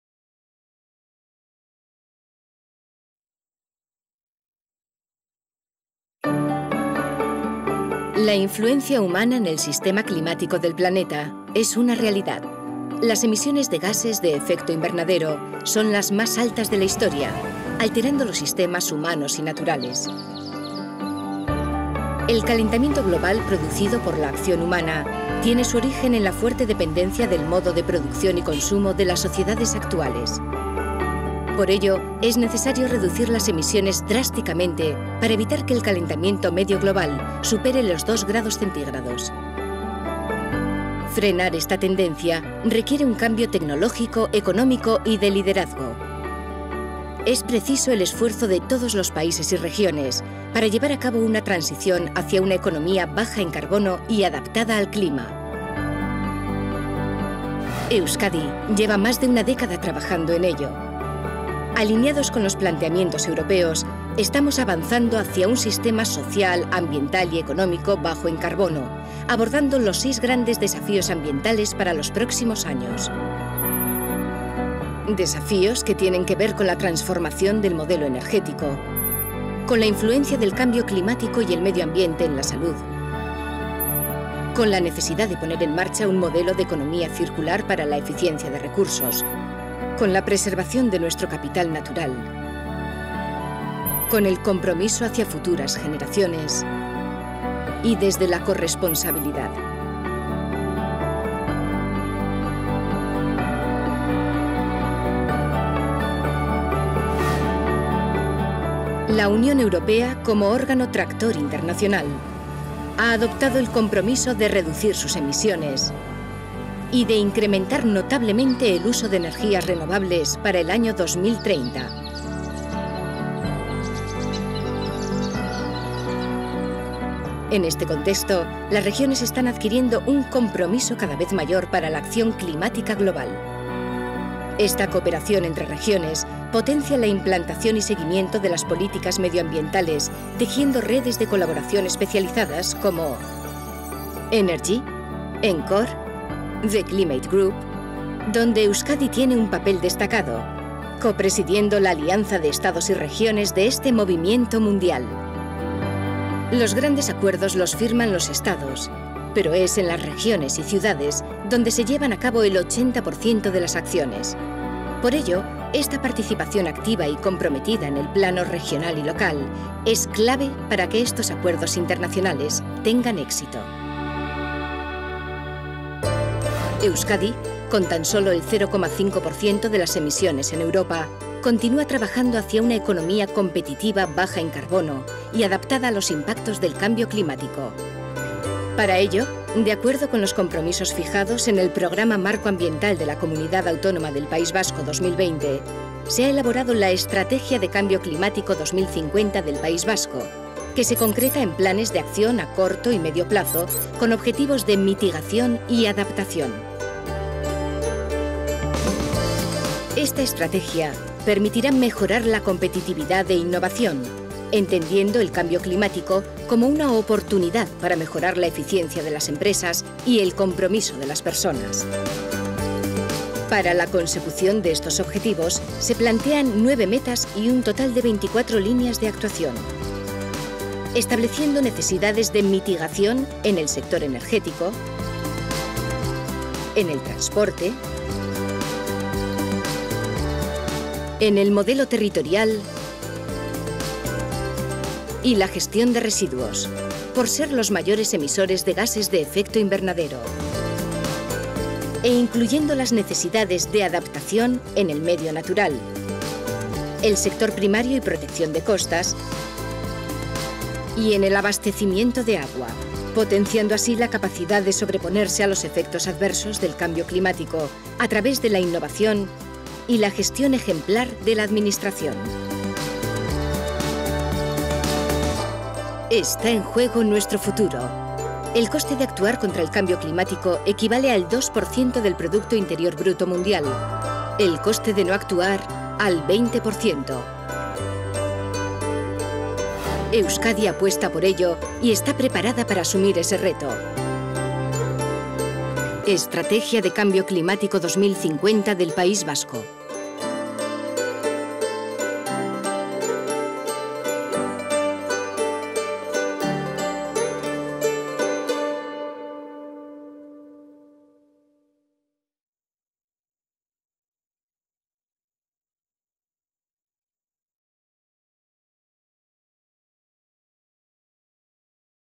EUSKADI, COMPROMETIDA CON EL CLIMA
La influencia humana en el sistema climático del planeta es una realidad. Las emisiones de gases de efecto invernadero son las más altas de la historia, alterando los sistemas humanos y naturales.

El calentamiento global producido por la acción humana, tiene su origen en la fuerte dependencia del modo de producción y consumo de las sociedades actuales.

Frenar esta tendencia requiere un cambio tecnológico, económico y de liderazgo. Es preciso el esfuerzo de todos los países y regiones, para llevar a cabo una transición hacia una economía baja en carbono y adaptada al clima. Euskadi lleva más de una década trabajando en ello.